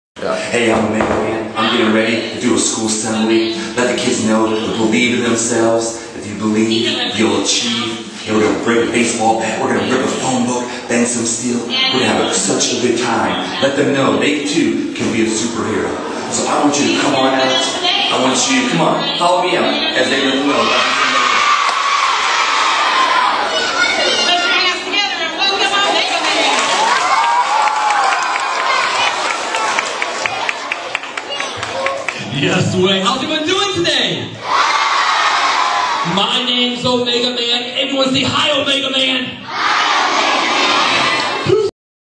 speaks on bullying in schools
Omega-Man getting ready to speak to students about anti-bullying